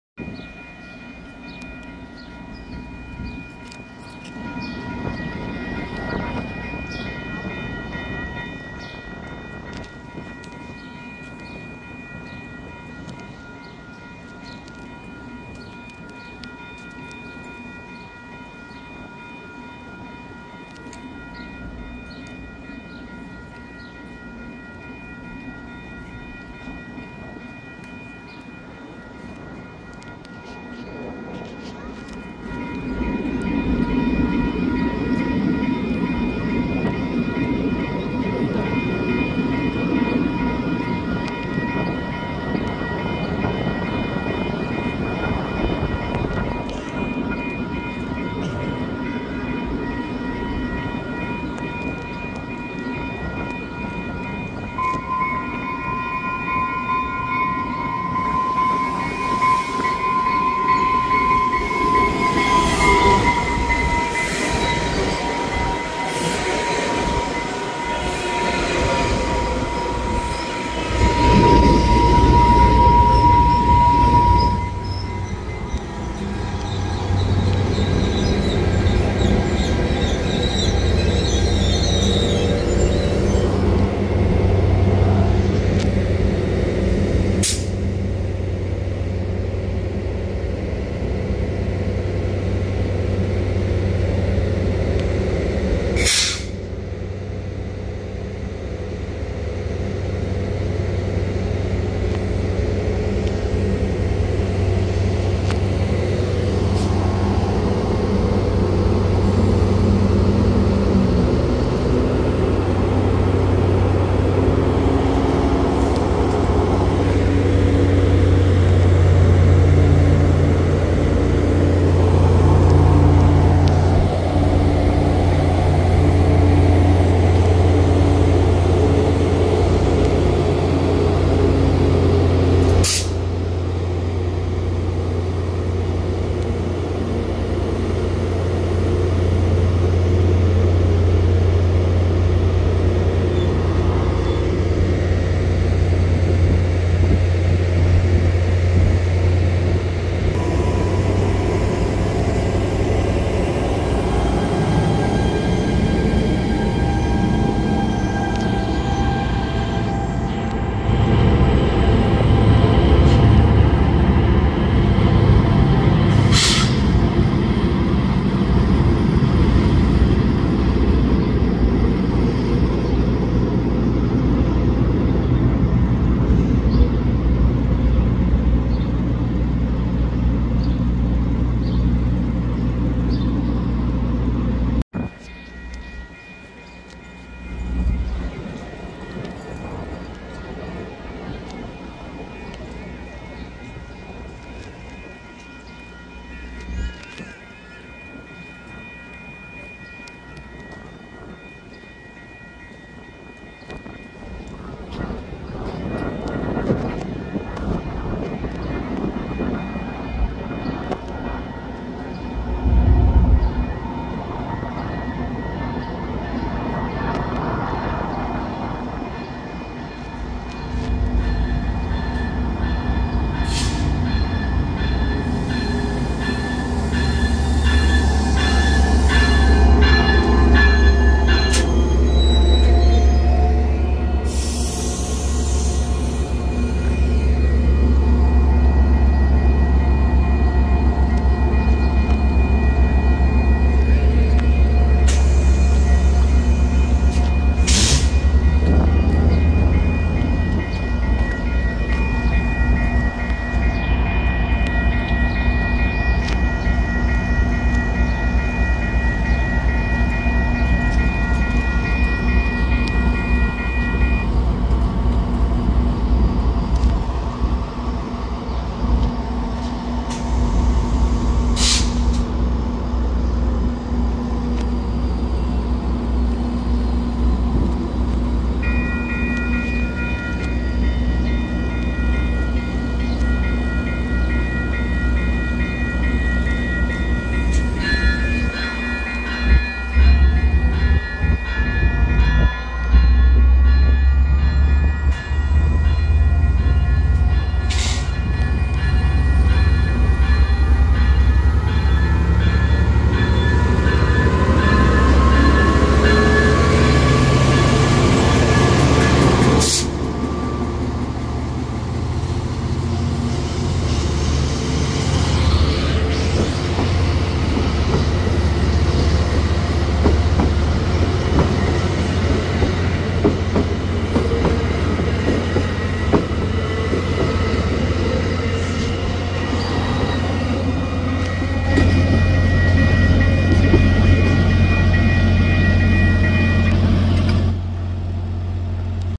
EXO 183 And 188 At Gare Parc (F59PHI with weird and unusual bell, must take a listen!)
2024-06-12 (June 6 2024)

Went back to gare parc and caught EXO 183 which had 2003 and 1361. Average 183 consist nowadays. This would be a consist usually on 185 or 187. And 188 with the usual multilevel consist which had 1328 (Heard this train at 7:21 AM headed to Saint Jerome so I knew it had 1328) and decided to see it. And by suprise, it does not have a ekyrail anymore. It has a very weird Graham electric bell. I think this is because EXO is rebuilding theyre F59PHI's and maybe they will use Graham Bells instead of Transtronic and Ekyrail. Last time I saw 1328 was at Jarry park but the bell was not on so I did not hear it. And apparently, 1323 is now in the shops to be rebuilt.

Note: this video was made by me and it is not stolen, Thank you.